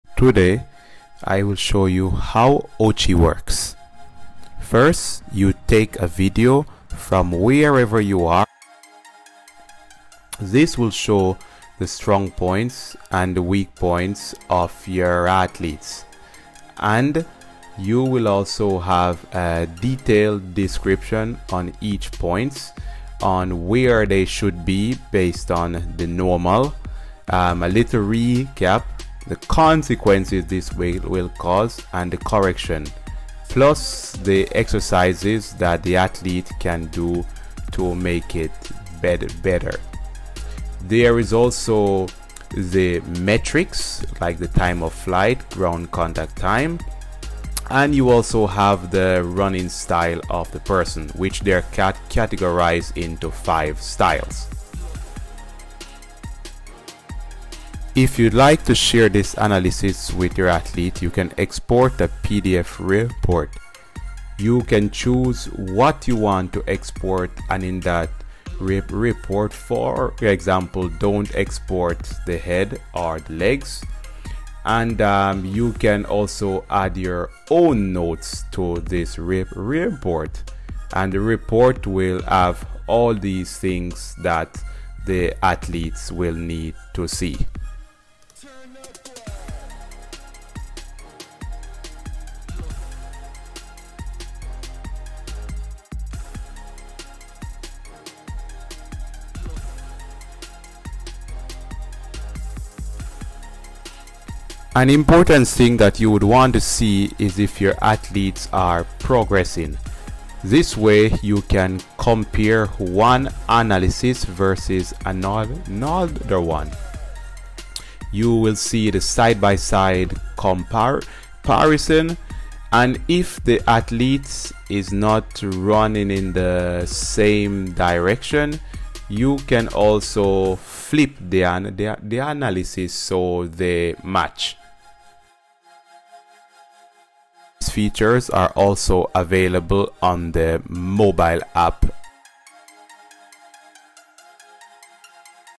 Ochy: Revolutionizing Biomechanics Analysis in 60 Seconds! | Instant Athlete Feedback
"Discover how Ochy's cutting-edge AI technology transforms biomechanics assessment with just a smartphone camera.

In this video, we demonstrate:

How Ochy works: Upload a video and receive a detailed analysis in just 60 seconds.
Comprehensive insights: Identify strong and weak points of athletes with precise metrics and running style evaluations.
Easy reporting: Export a detailed PDF report and share it effortlessly with your athlete.
Comparative analysis: Track progress by comparing different assessments over time.
Mobile convenience: Enjoy all these features on the go with our mobile app.
Don't miss out on how Ochy is bringing lab-quality biomechanics analysis into the palm of your hand.